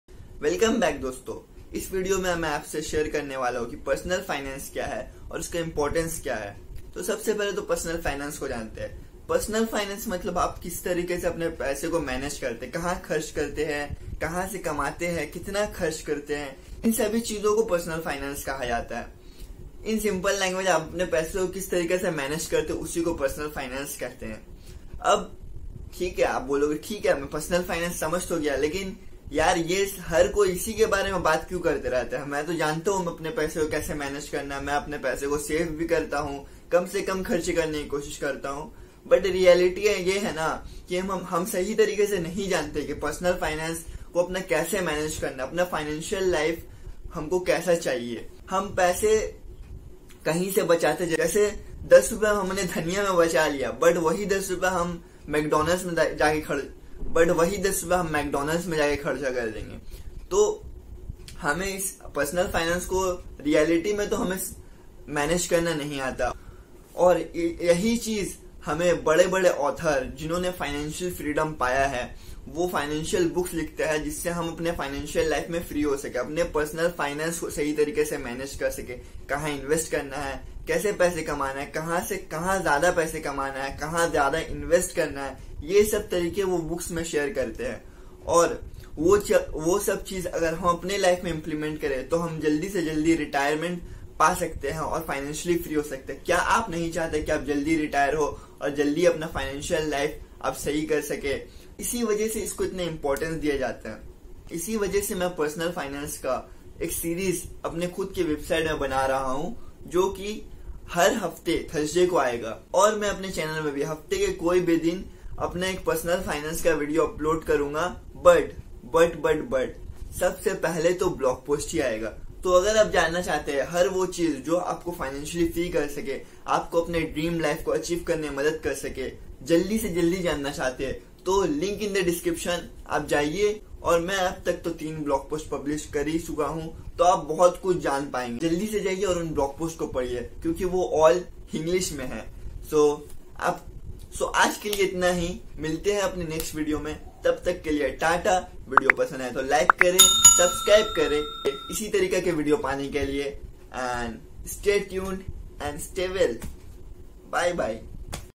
What is Personal Finance and Why it is Important?
Akhir Yeh Personal Finance hota kya hai ?

Aap apnai paisai ko kis tarekai se istaimal karte hai kaha invest karte ho kaha kharch kate ho taake ap financial free ho paaoo usse ko personal finance kahte hai.

Aaasaan bhasa mai ap apne paisai ko kis tarekai se manage karte ho wahi personal finance kahlata hai.